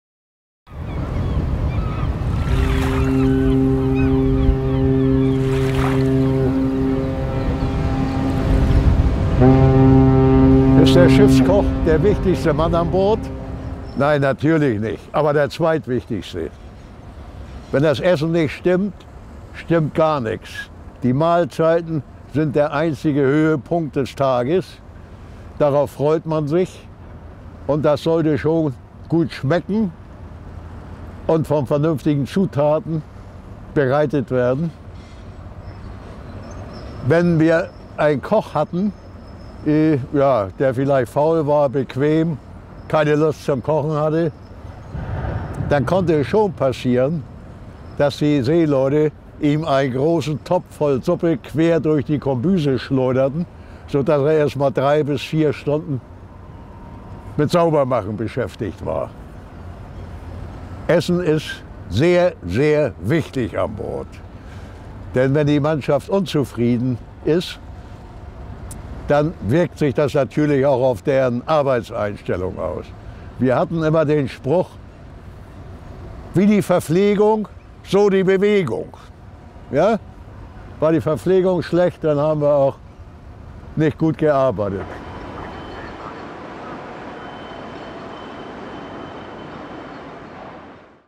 Käpt'n Schwandt: Ist der Schiffskoch der wichtigste Mann an Bord?
Ist der Koch der wichtigste Mann an Bord? Kapitän Schwandt, der alte Seemann aus Hamburg, hat eine klare Meinung. Mehr vom "Kult-Käpt´n" (Hamburger Morgenpost)